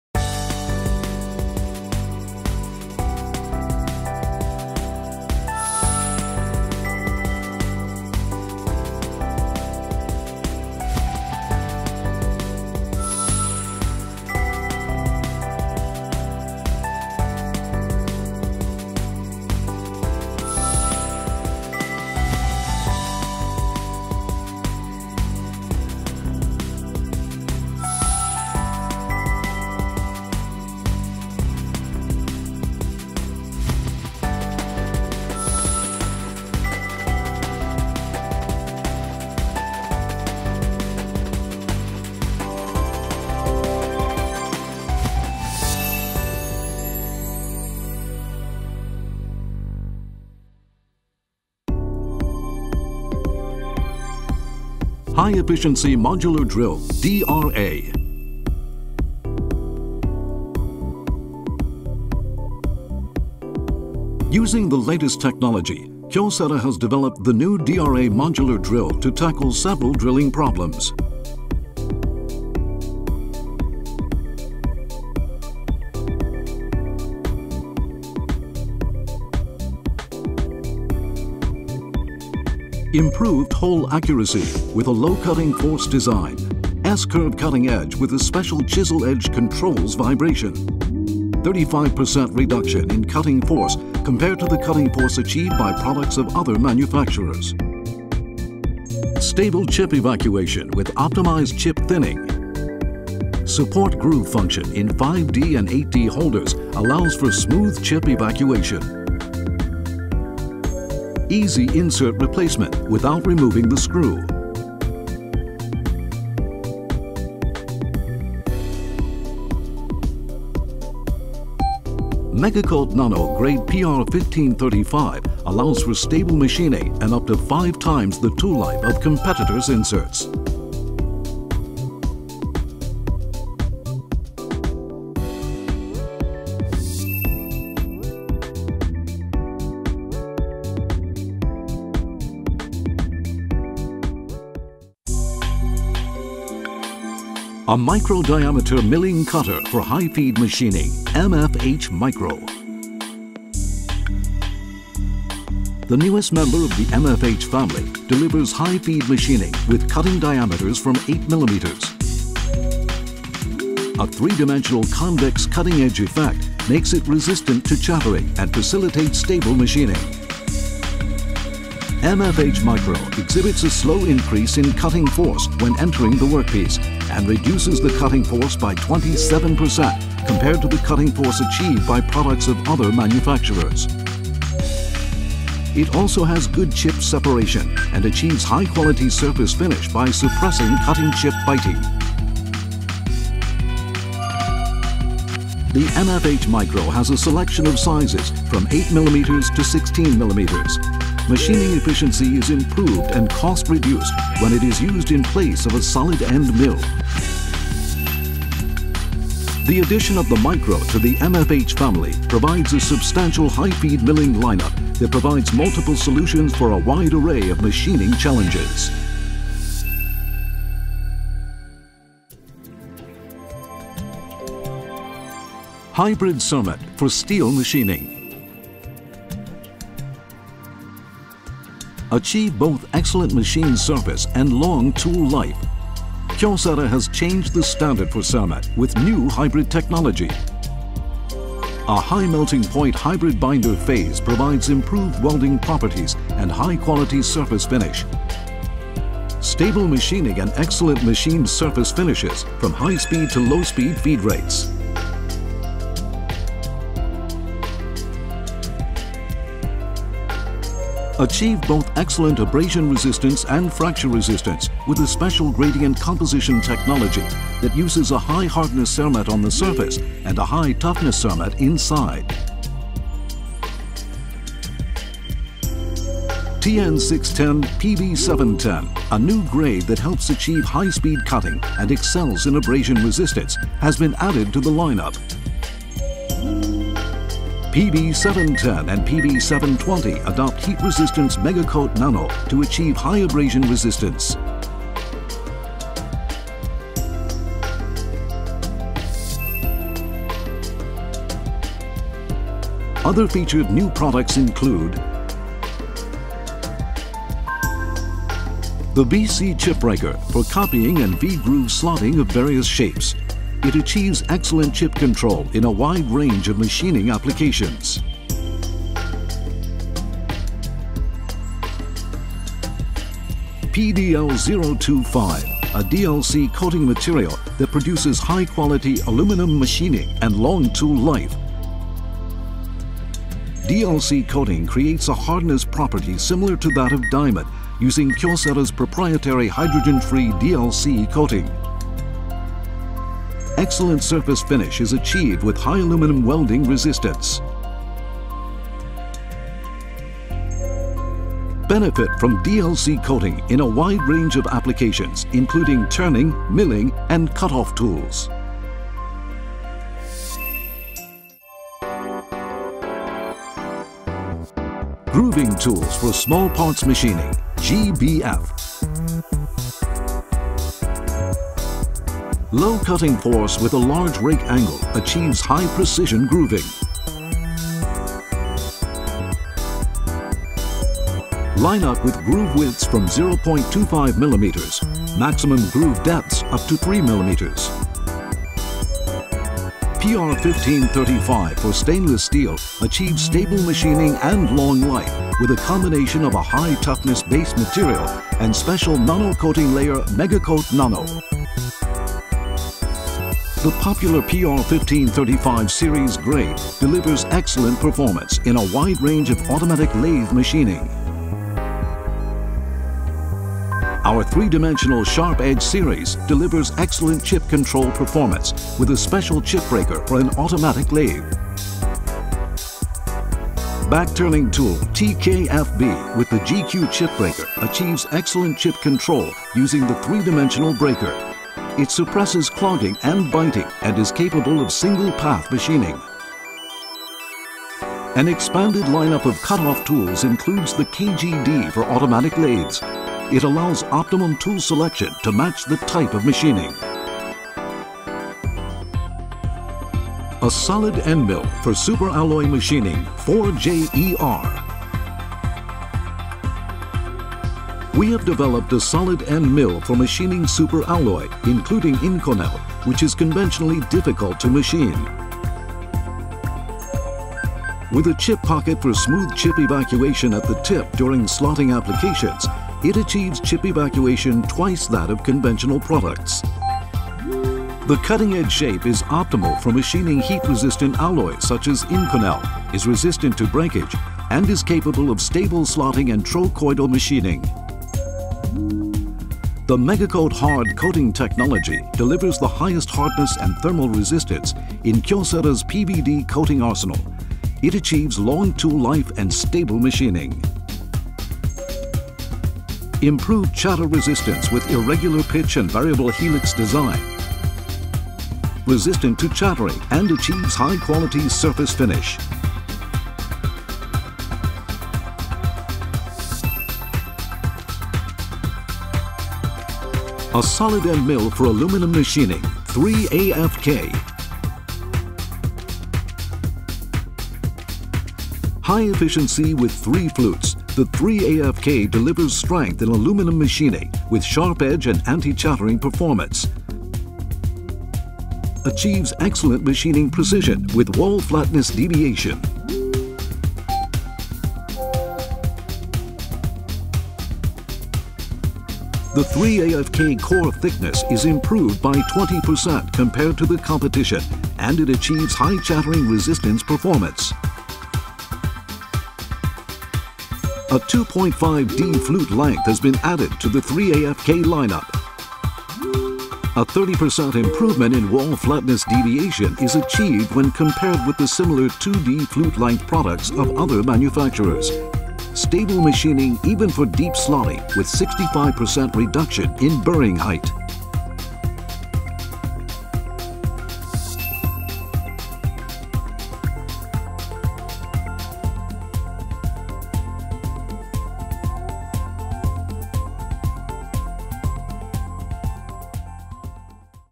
KYOCERA New Products 2016
Brand new product offerings featuring the new DRA high efficiency modular drills, MFH-Micro high feed mills, and insert grade PDL025 for aluminum machining among various other tooling lineup expansions. Check out the new lineup.